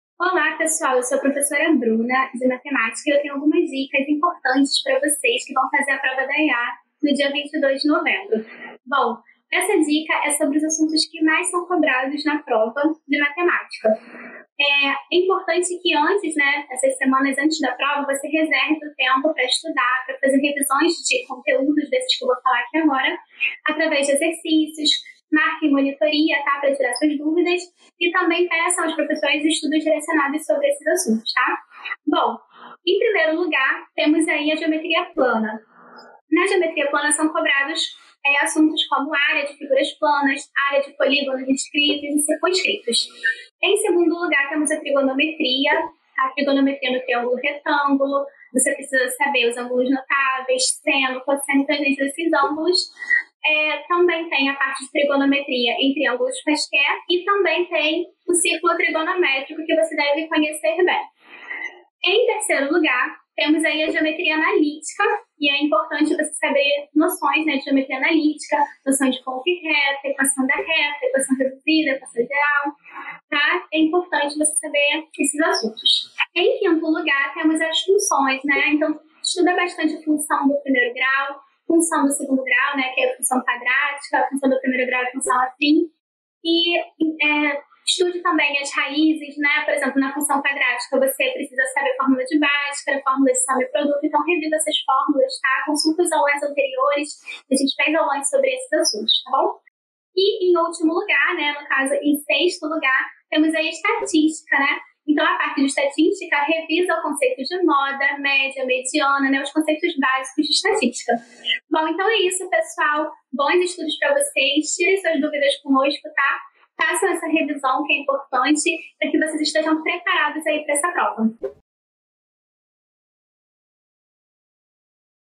Dicas Conteúdo prova EEAR - Professora Bruna (Matemática)
CHEGOU O DIA DA PROVA!

Winners, boas dicas da nossa professora Bruna para a prova desse Domingo!!!⠀
Estamos juntos!!!⠀